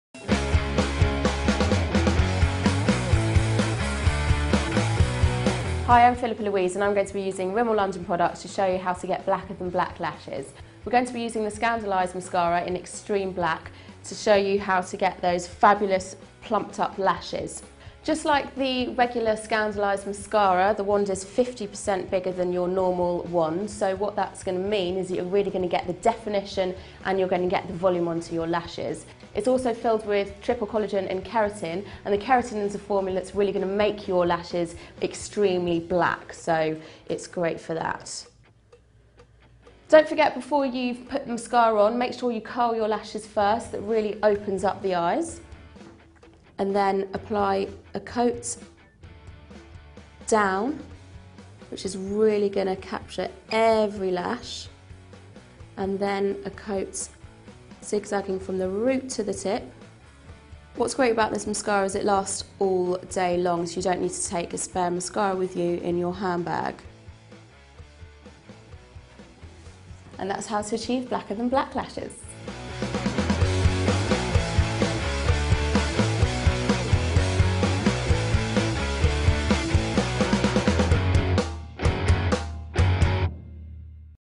How to get blacker than black lashes by Rimmel London
Allow Rimmel London to show you how to achieve blacker than black lashes with Scandaleyes Extreme Black Mascara.